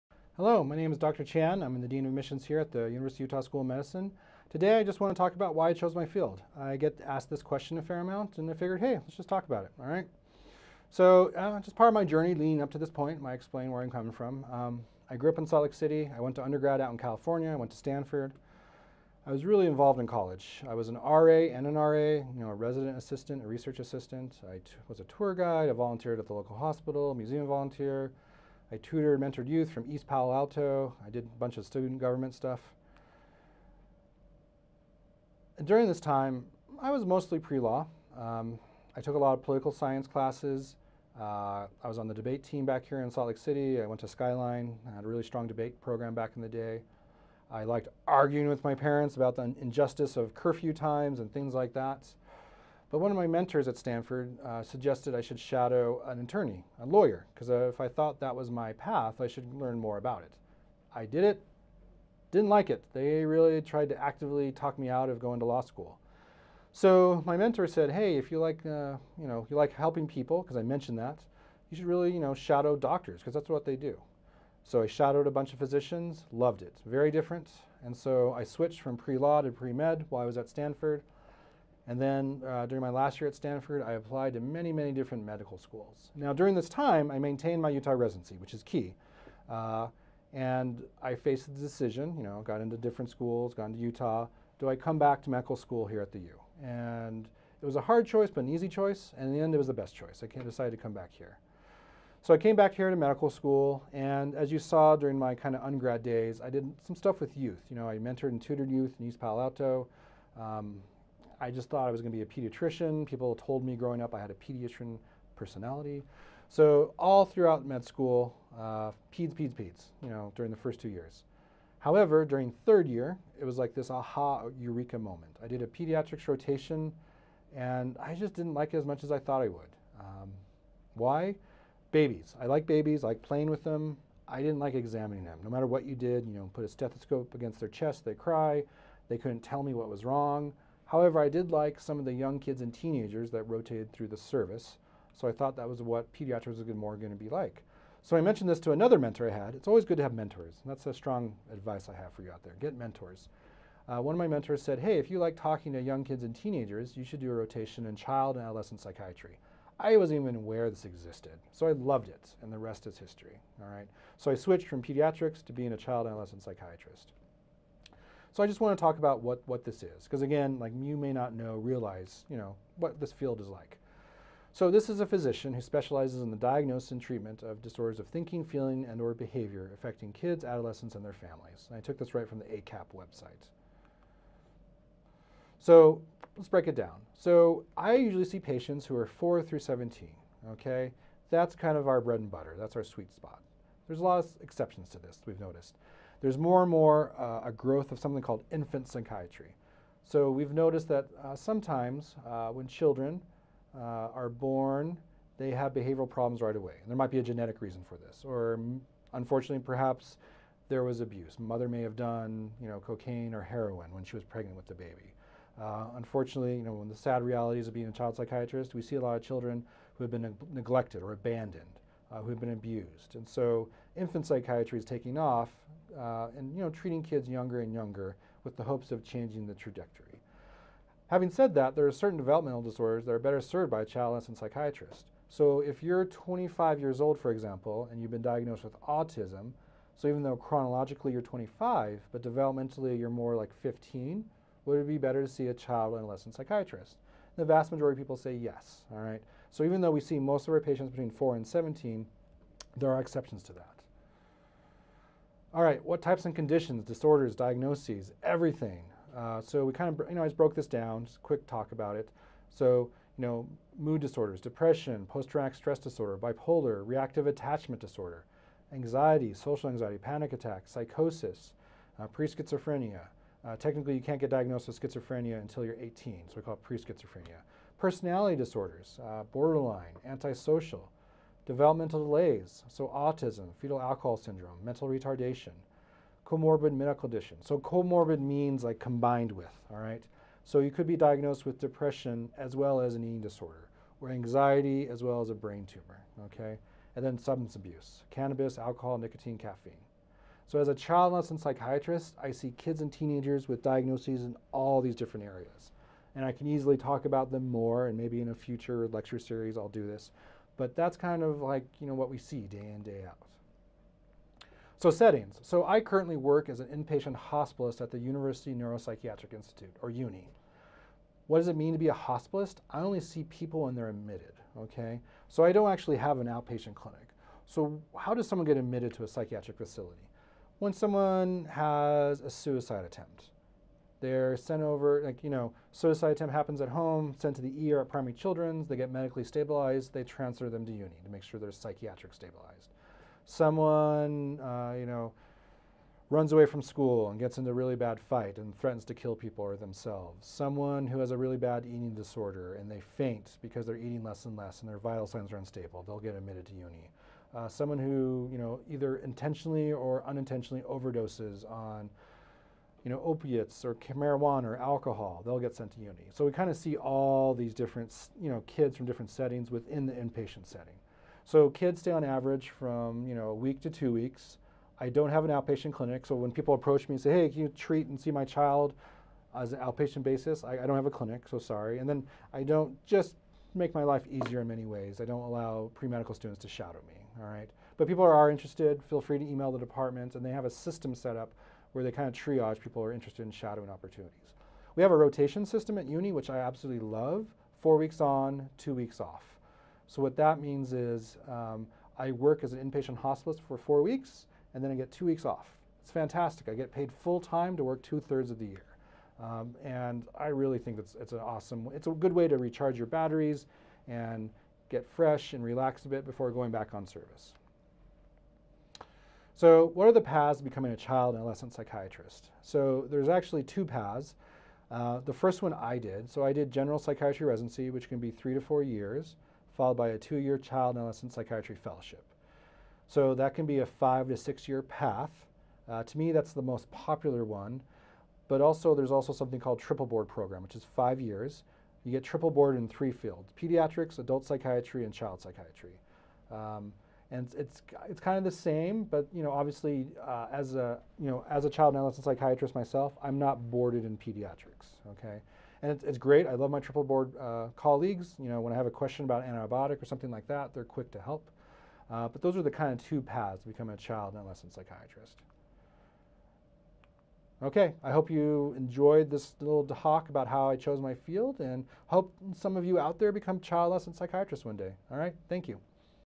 Why I Chose My Field
Why did Dr. Chan choose his field of medicine?  Take a look and find out!